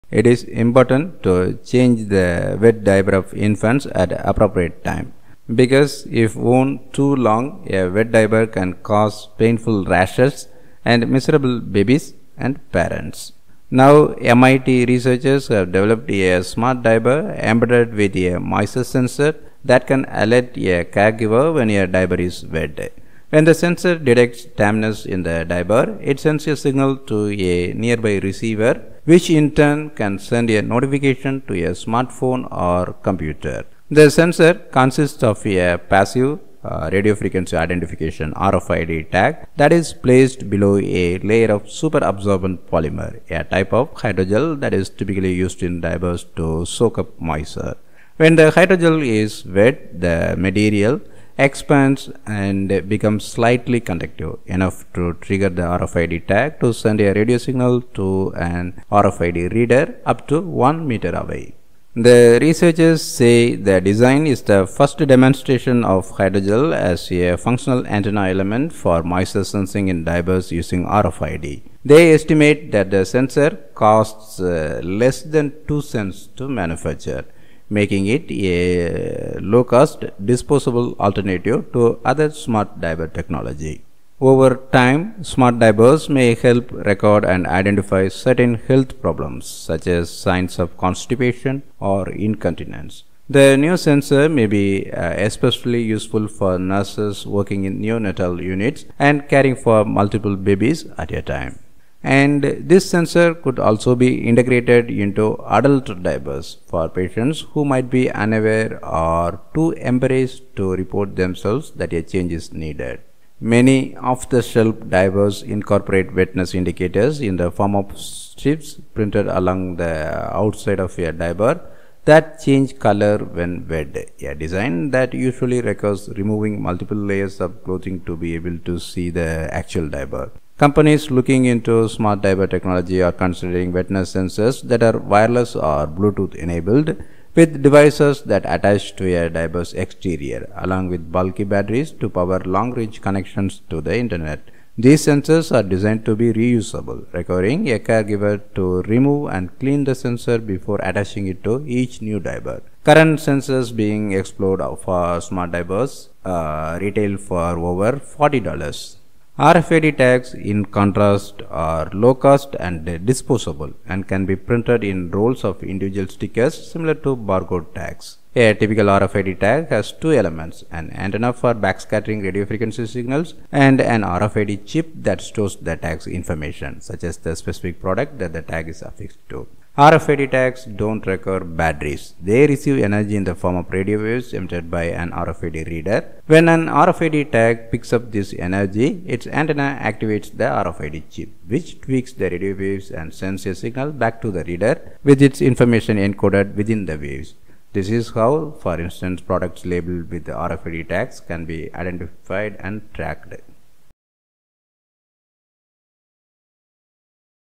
Low-cost “smart” diaper can notify caregiver when it’s wet
MIT researchers have developed a “smart” diaper embedded with a moisture sensor that can alert a caregiver when a diaper is wet. When the sensor detects dampness in the diaper, it sends a signal to a nearby receiver, which in turn can send a notification to a smartphone or computer.

The sensor consists of a passive radio frequency identification (RFID) tag, that is placed below a layer of super absorbent polymer, a type of hydrogel that is typically used in diapers to soak up moisture. When the hydrogel is wet, the material expands and becomes slightly conductive — enough to trigger the RFID tag to send a radio signal to an RFID reader up to 1 meter away.